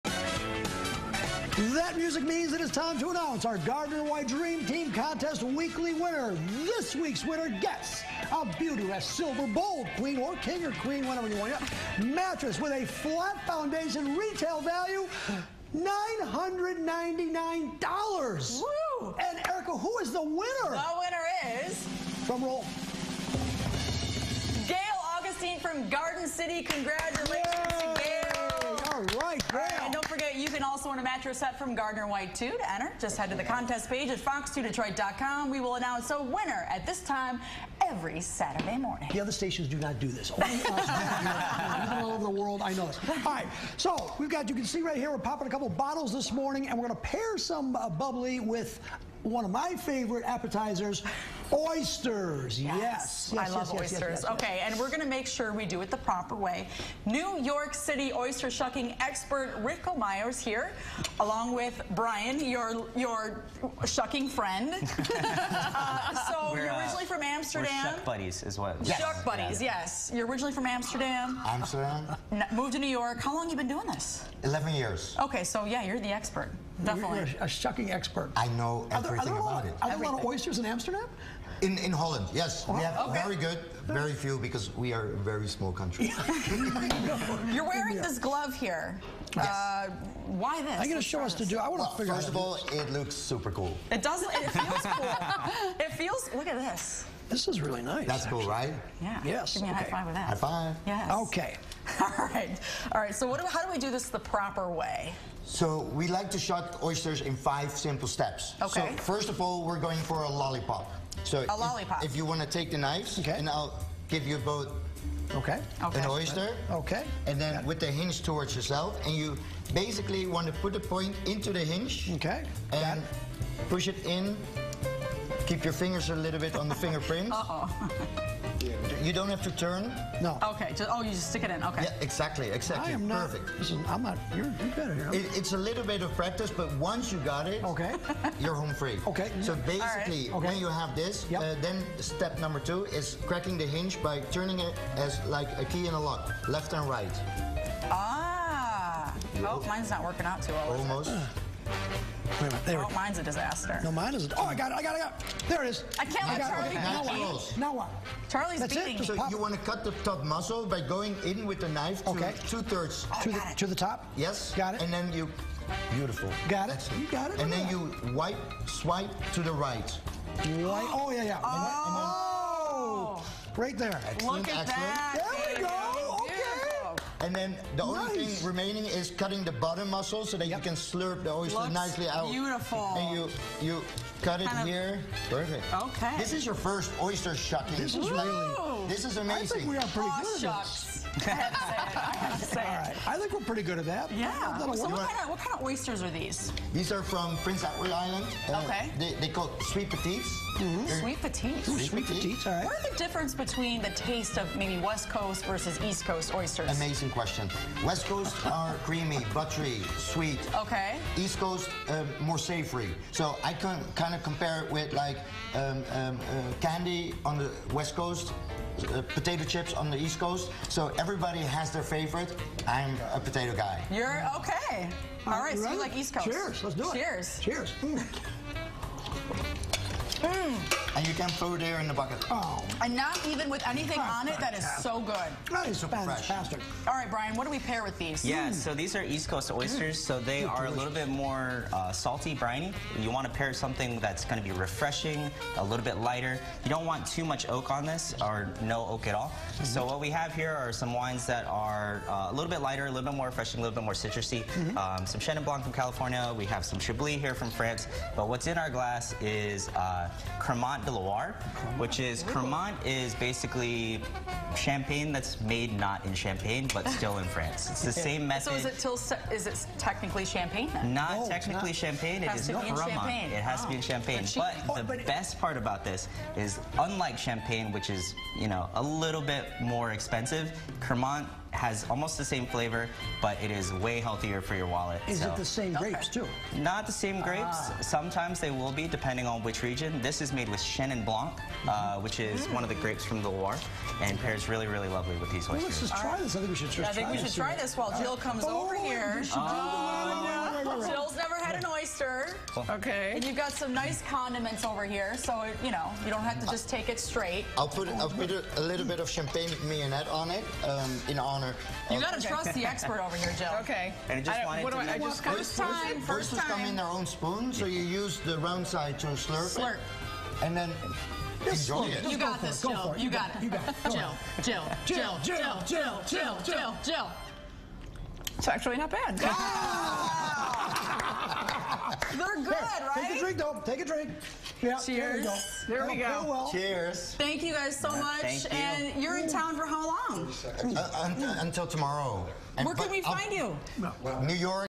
Oyster Chef Rifko LIVE on Fox 2 Detroit: Shucking Oysters with the Pros Oysters XO Live
Oysters are a popular summer-time meal but there's a trick or two to shucking and eating them right.

New York City-based oyster expert Rifko Meier, of Oysters XO, is in town this weekend for special appearances at Fram in Hazel Park. Meier and Bryan Lamorena, the beverage director at Frame, dropped by the Fox2 Kitchen to teach us and Fox2 anchors Charlie Langton and Erika Erickson how to shuck the right way.